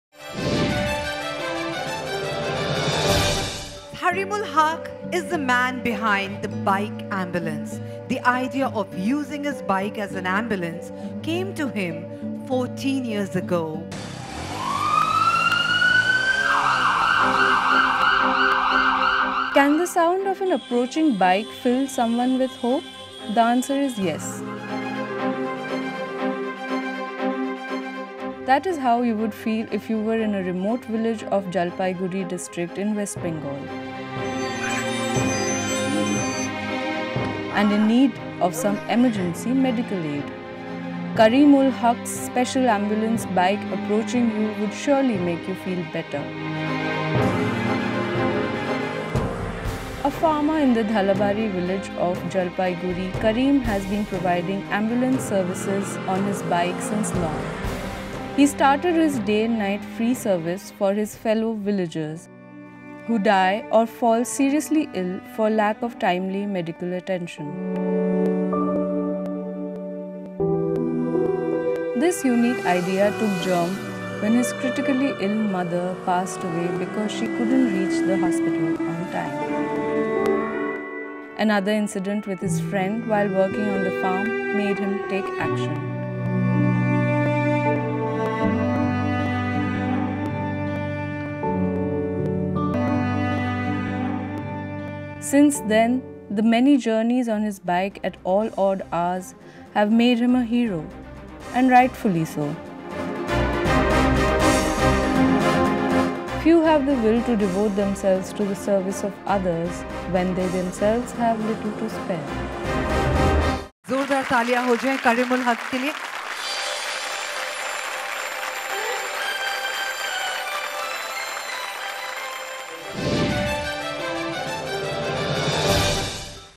Bike Ambulance Dada | Karimul Haque | The Shoorveer Awards 2017
After his mother’s death due to lack of ambulance facilities in Dhalabari Village, Karimul decided that he will not let another person die the same way. He ferries the weak and sick to the hospital on his bike, be it night or day, and has become the saviour of people in 20 villages in the Jalpaiguri district of West Bengal.